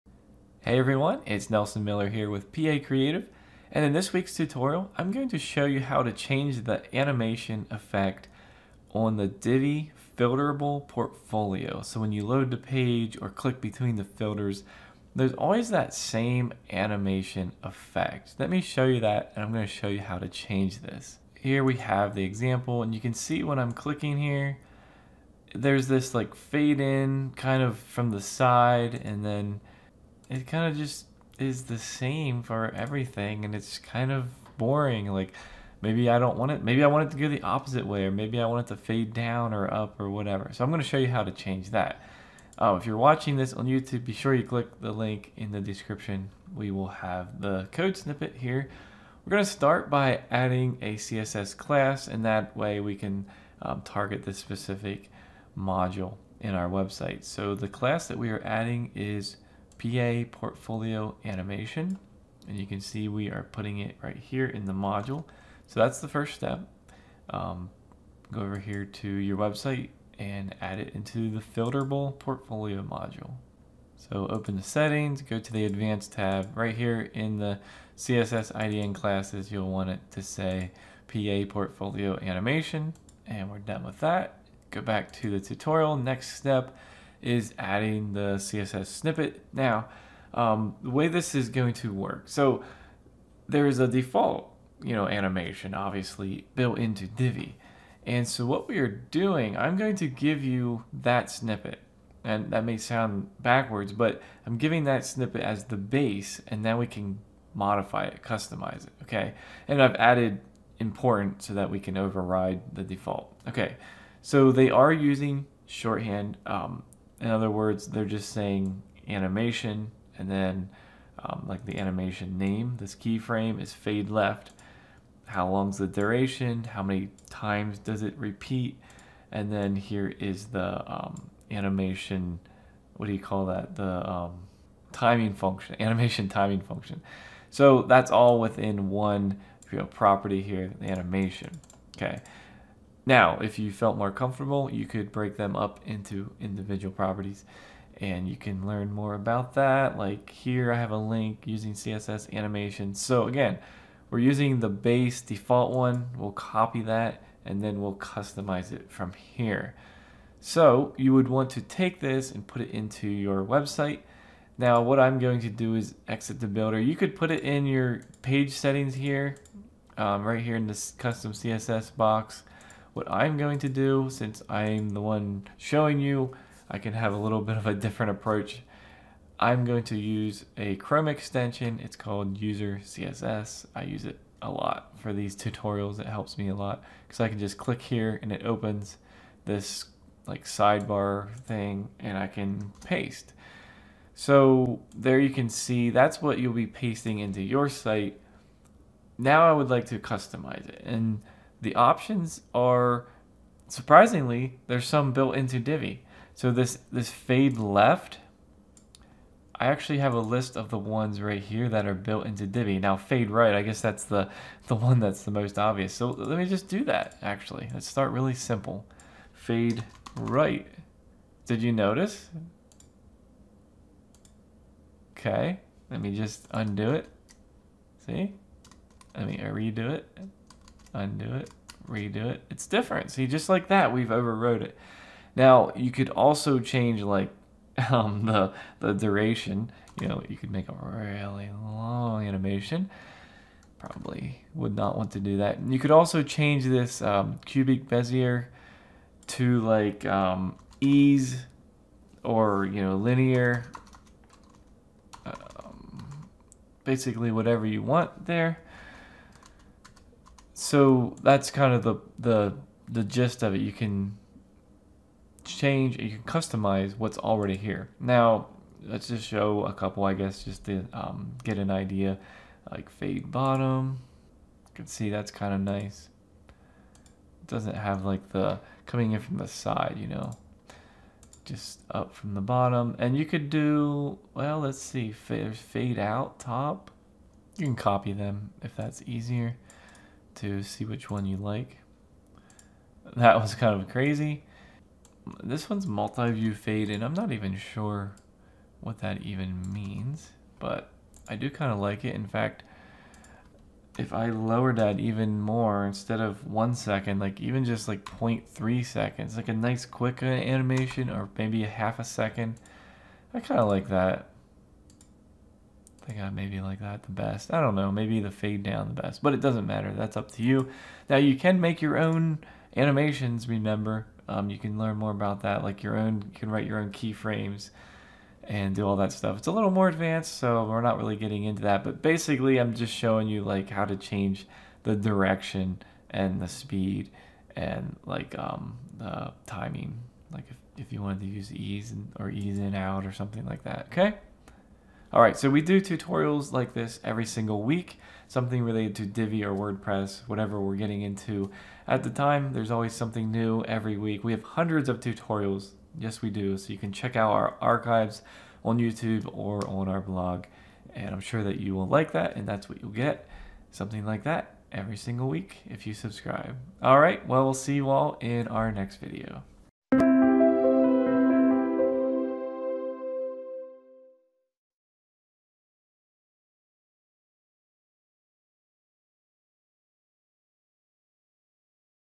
How To Change The Divi Filterable Portfolio Animation
In this tutorial I will show you how to change the Divi Filterable Portfolio animation when switching between filters.

The Divi Filterable Portfolio module is popular for its category tabs which allow people to filter the items shown, but if you ever used it you probably noticed the animation. When the module initially loads or when you switch to different filters, the items in the portfolio animate from left to right. While this is fine, it’s also a bit odd that there are no settings to change the animation effect. So in this tutorial, I will show you how to use CSS to change the Divi Filterable Portfolio animation affect when switching between filters.